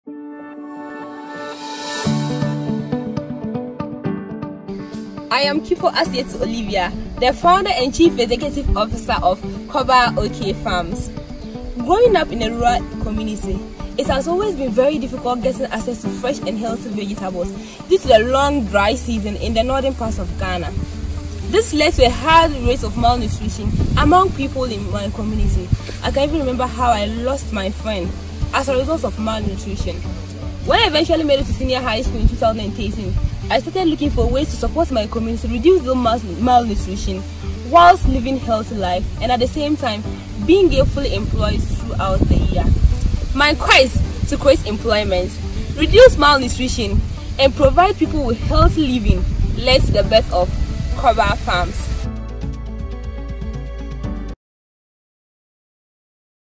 Aseitu Olivia | Anzisha Prize Fellowship
Aseitu Olivia Kipo, 22
GHANA
Founded Kobaa-Ok in 2019 and has created jobs for 8 full-time and 8 part-time employees
Sector: Agriculture Sector